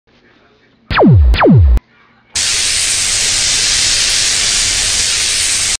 Laser/Lightning Gun Effect [Test]
I'm practicing for a film my friends and I are doing.

[Edited in Adobe Premiere Elements 7]